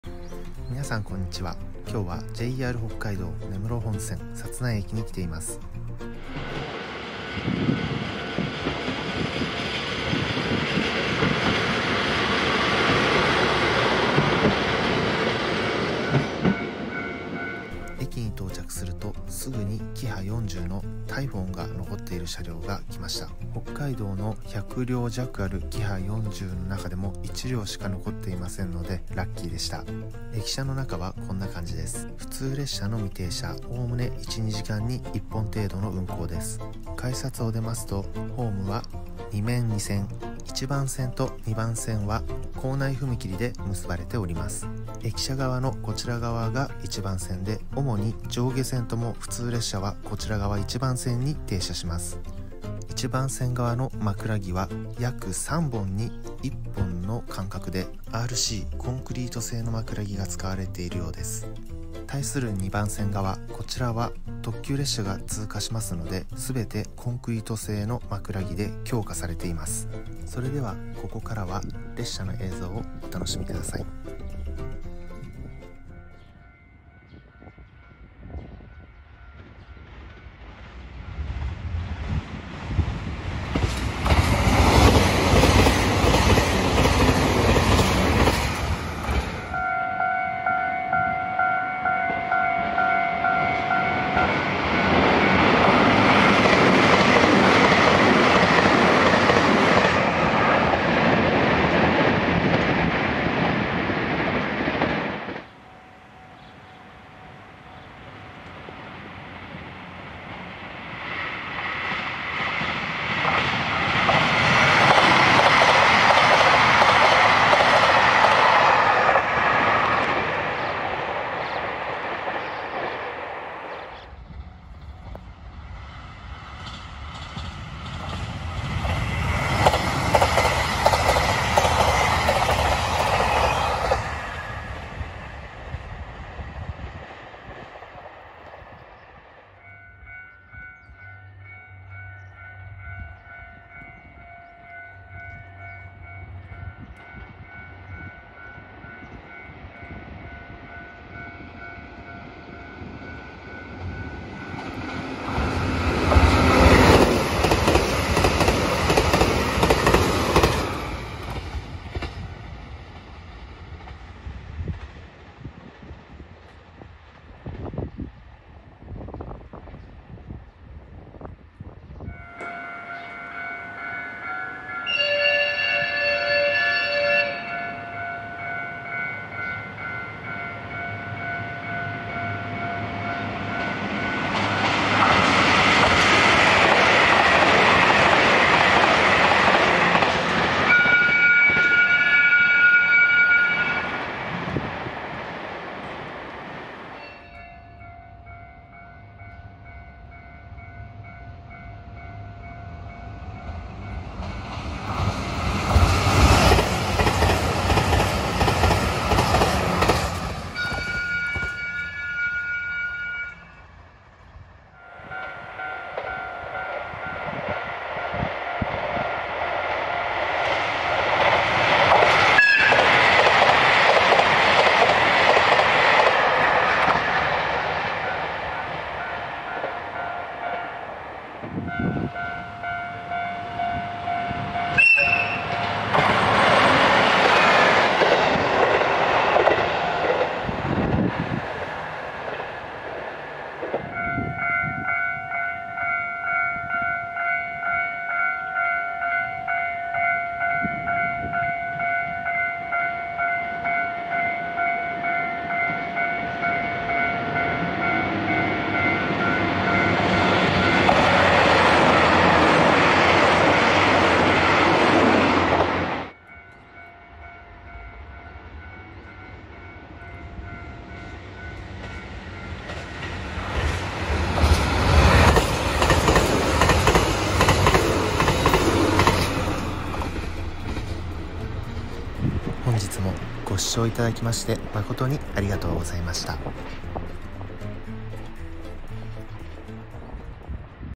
JR北海道 根室本線 札内駅 周辺
JR北海道 根室本線 札内駅周辺で撮影しました。
警笛が多く鳴りますので音量にご注意ください。